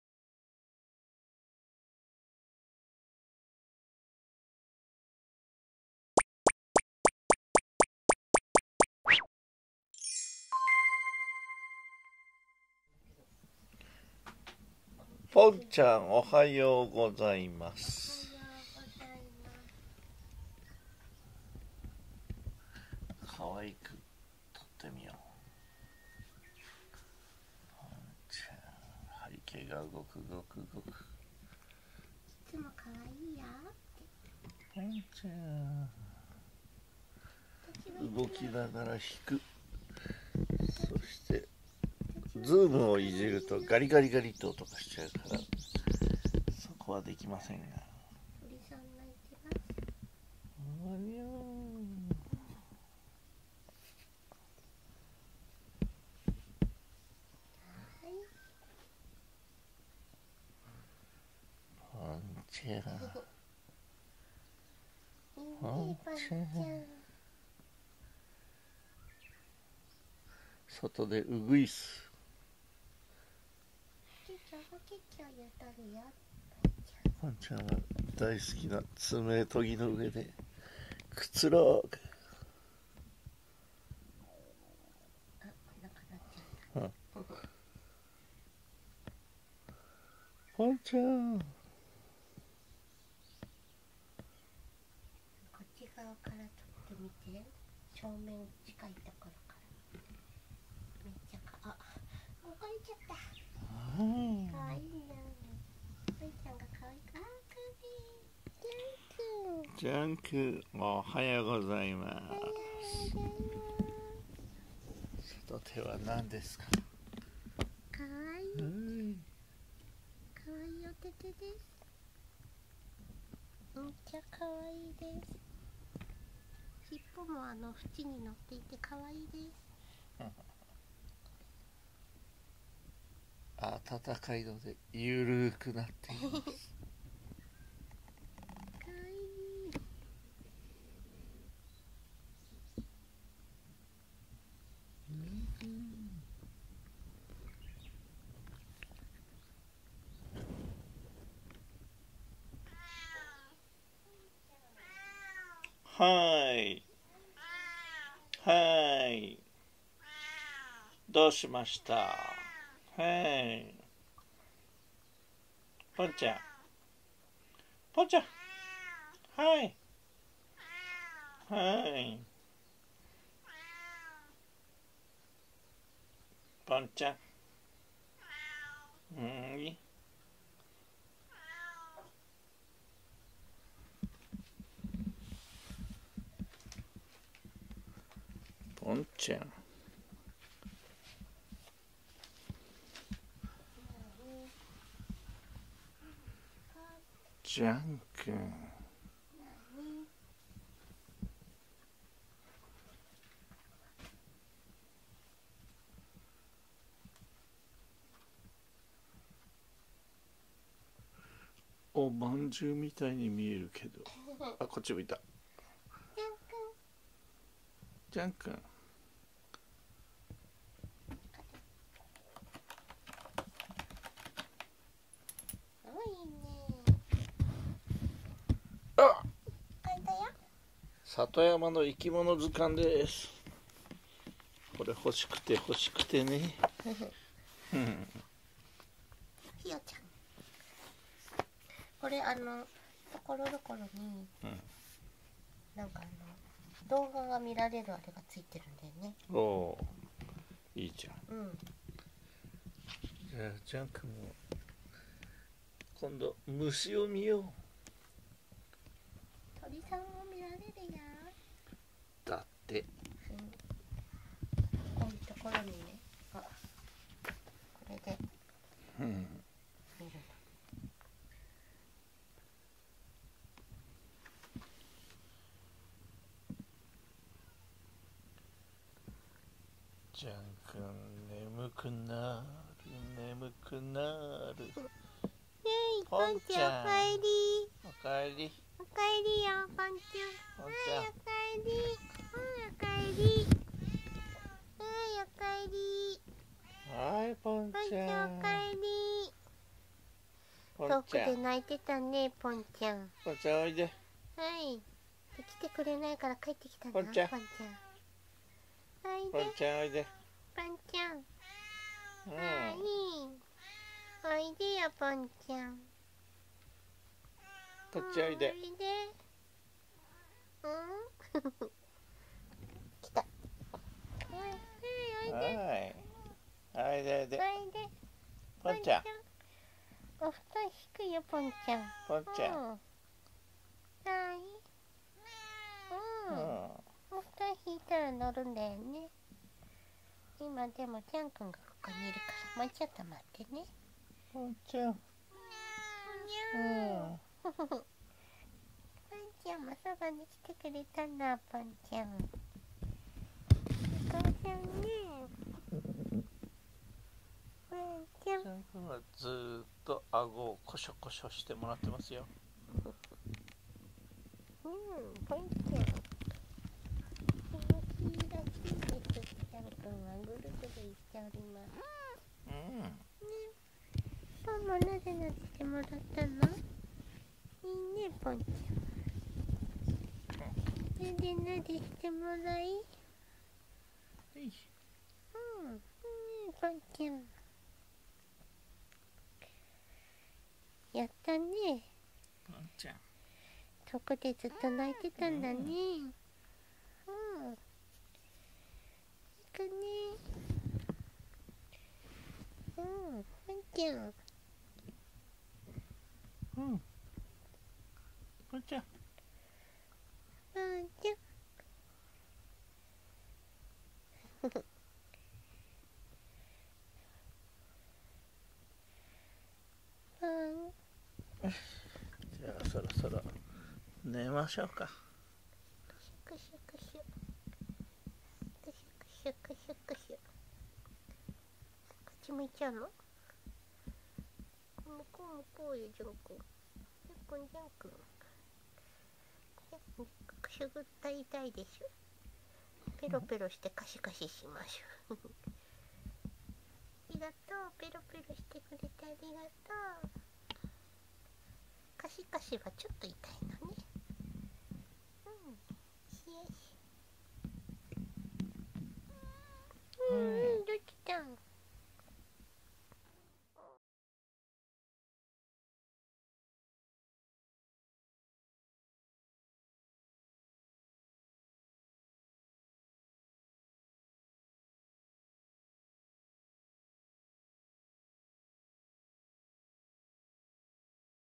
ポンちゃんがよく鳴く様になったのは信頼の証かな？【Jean & Pont 2816】2022/4/12 保護猫ジャンけんポン
不意に鳴き始めるのがポンちゃんです。
あまりに急に鳴くのでパパもママも何がして欲しいのかよくわかりません。
多分ですがふと寂しくなったからじゃないかと思います。
ちなみにテレビ番組から出版された里山の生き物図鑑を買いました！